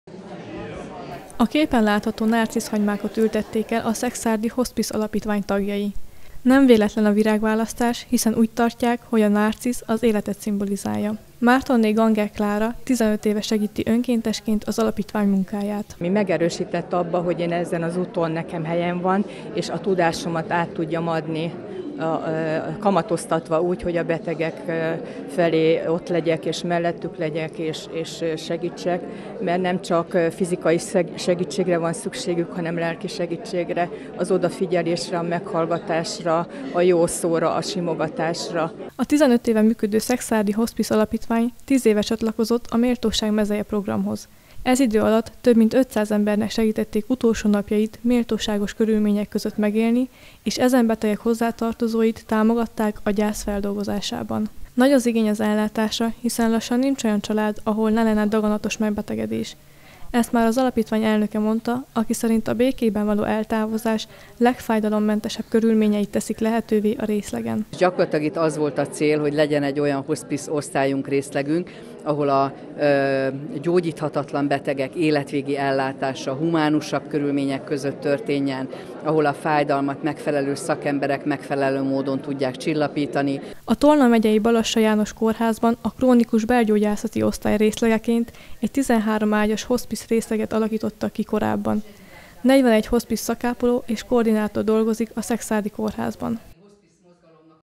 Nárciszültetéssel egybekötött Hospice Világnapi rendezvény - Szekszárd 20191010
Nárciszültetéssel egybekötött Hospice Világnapi rendezvényt szervezett a szekszárdi Balassa János Kórház “Méltósággal az út végén” nevű alapítványa. 10 éve csatlakozott az alapítvány a Méltóság mezeje országos programhoz. Ez idő alatt közel száz önkéntes segítette azokat a daganatos betegeket, akik hosszabb időt kénytelenek az intézményben tölteni.